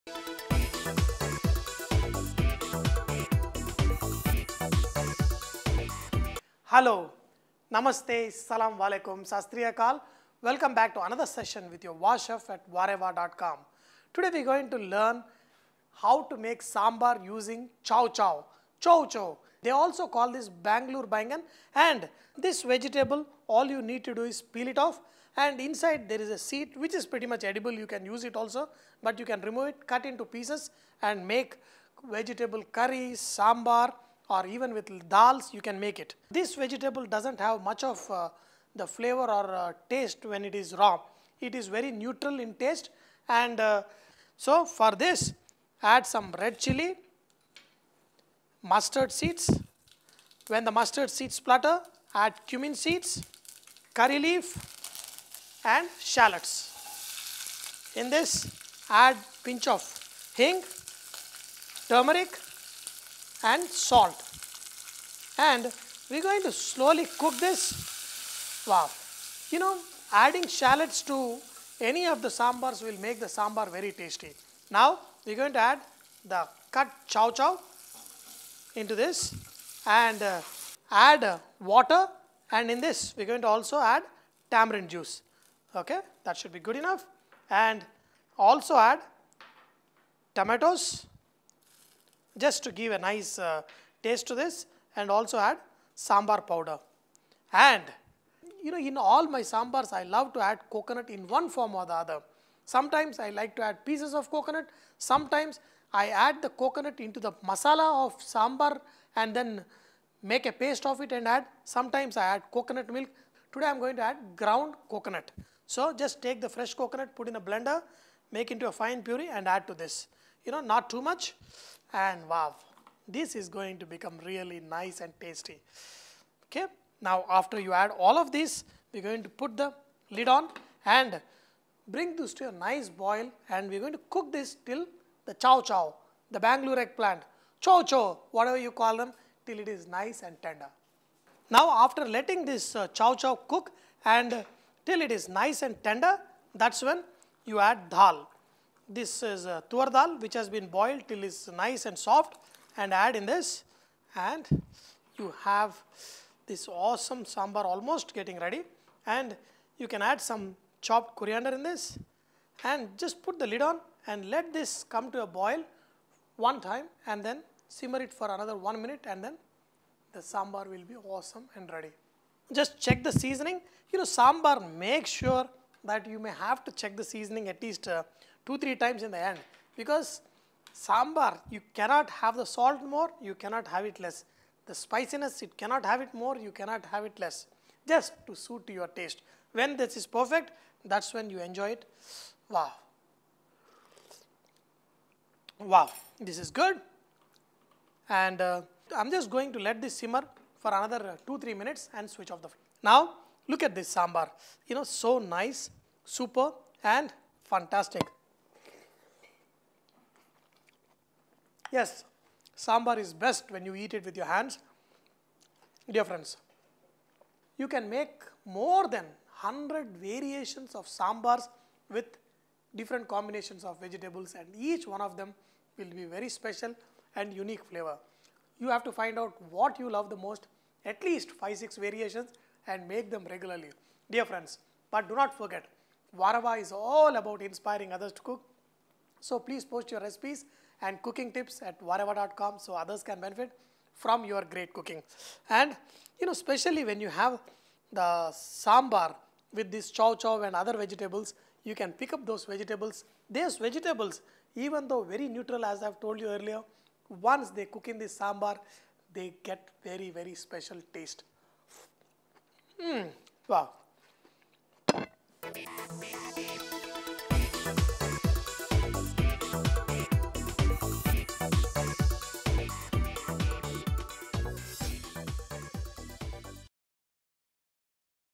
Chow Chow Sambhar - By VahChef @ VahRehVah.com
Chow chow sambar
Chow chow sambar  is a traditional sambar preparation popular in and around the districts of Tamil Nadu. Sambar is generally prepared at every home with any vegetable, lentils and spices cooked together in a tangy soup. In this recipe, chow chow or chayote is added and cooked with lentil  to make an excellent dish. The dish is creamy, smooth and a brilliant combination of all flavours.

Ingredients:
Chow chow  - 1 nos
Red chillies  - 2-3 nos
Shallots  -  10-12 nos
Hing   - pinch
Tamarind juice - 1 cup
Tomatoes, sliced - 1 cup
Sambar powder - 3 tbsp
Ground coconut - ¼ cup
Tur dal (cooked ) - 2 cups
Coriander leaves - few
Methi seeds  - ¼ tsp

Method:
Peel the chow chow or Bangalore eggplant or chayote and cut into small cubes. Keep aside.
In a pan, heat oil. Add mustard and when they splutter, add cumin seeds, red chillies, curry leaves, hing and shallots. Sauté well for few minutes or till shallots turn slightly golden brown. Add turmeric powder, salt to taste and little water and mix well.

Now, add tamarind juice, sliced tomatoes, sambar powder and ground coconut paste. Allow the contents to boil well so that the chow chow is cooked and soft. Finally add cooked and mashed toor dhal. Medium the flame. Allow the contents to thicken. Garnish with chopped coriander leaves.